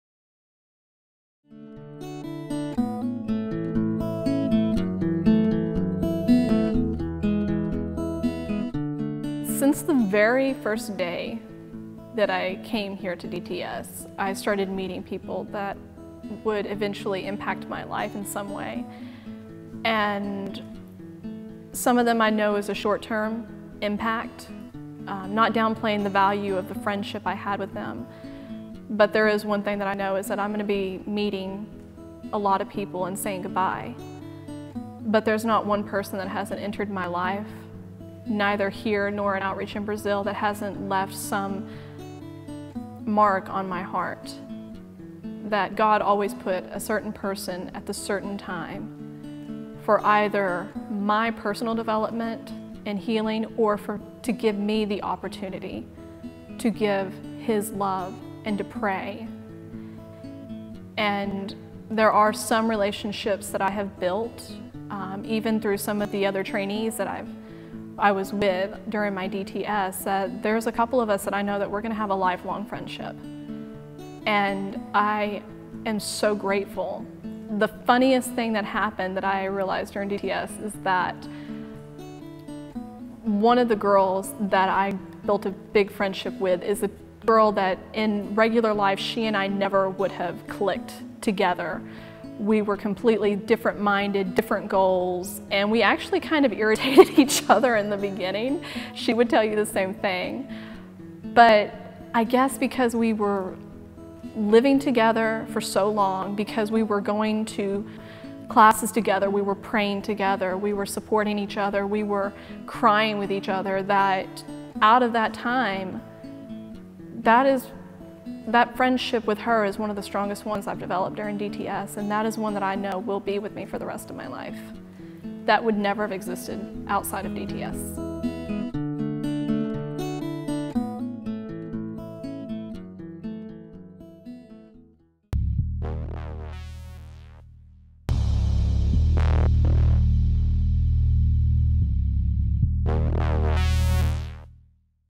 Karen Johnson Interview
Karen Johnson is a  YWAM Harpenden April 2014 Discipleship Training School graduate . Hear about her DTS experience and what she learned.